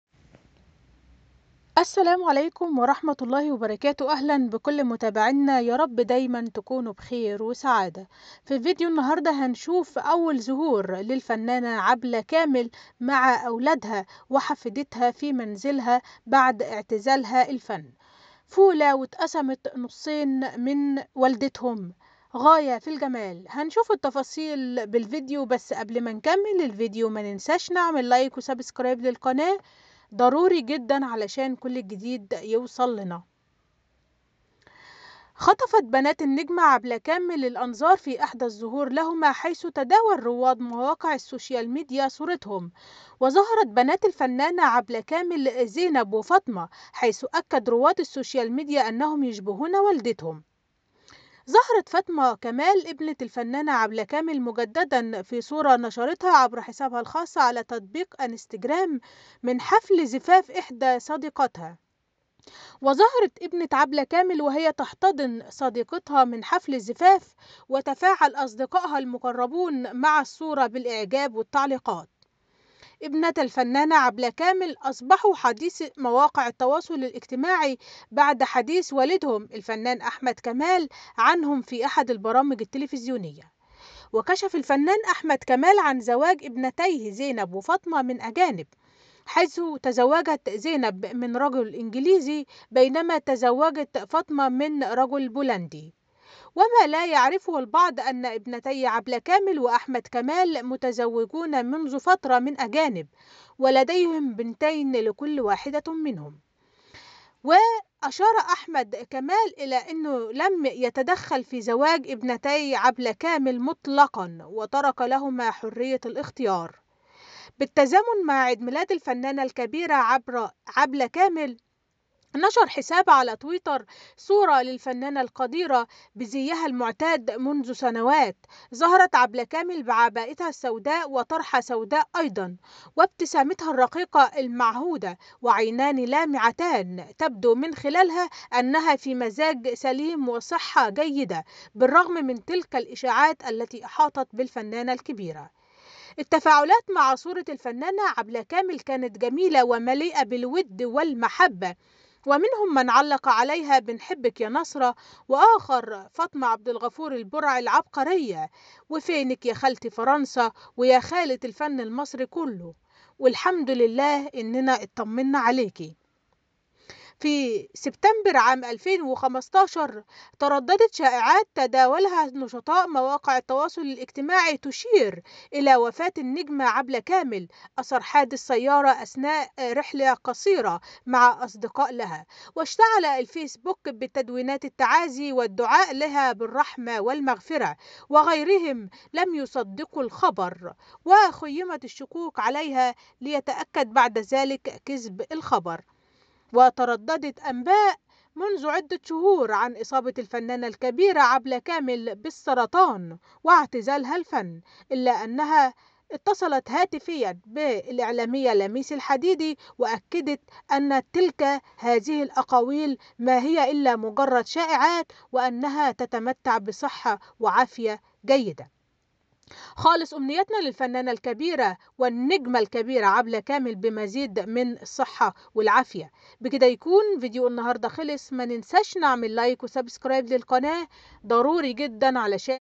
شاهد بالفيديو أول ظهور للفنانه عبله كامل مع اولادها وحفيدتها بعد اعتزالها فى منزلها غايه فى الجمال ؟
فى هذا الفيديو شاهد شاهد بالفيديو أول ظهور للفنانه عبله كامل مع اولادها وحفيدتها بعد اعتزالها فى منزلها غايه فى الجمال ؟